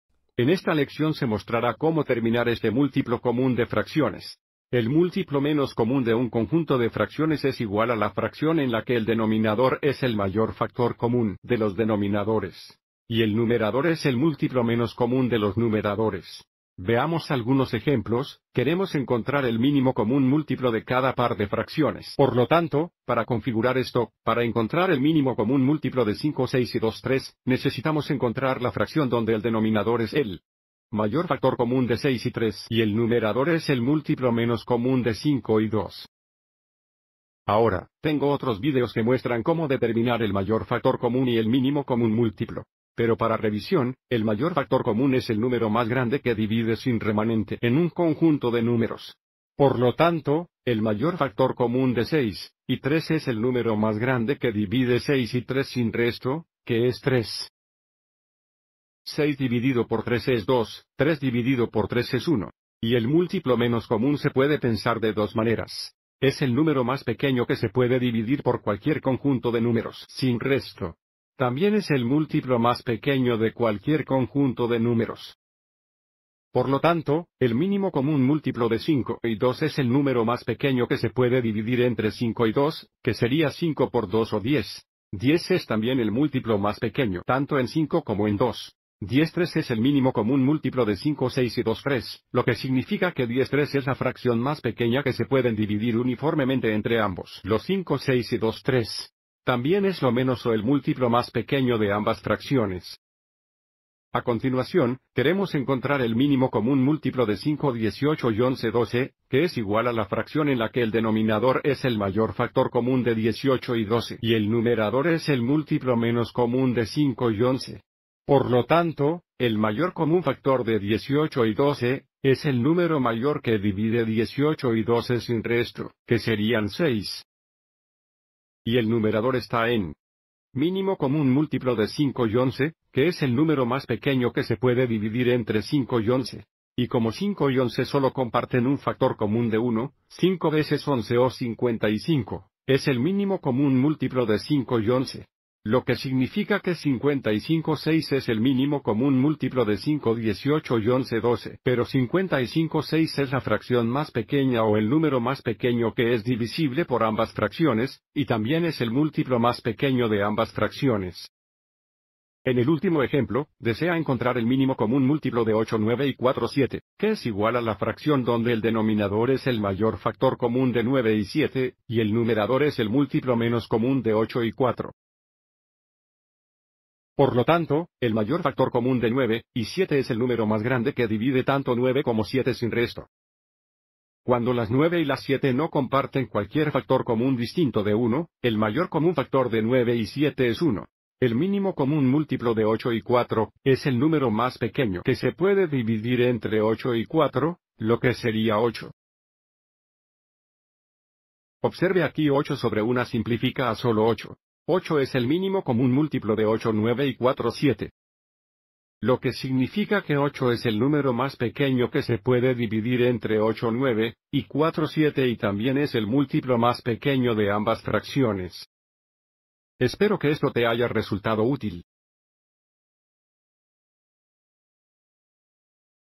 Hallar el mínimo común múltiplo de fracciones
Este vídeo explica cómo encontrar el MCM de pares de fracciones.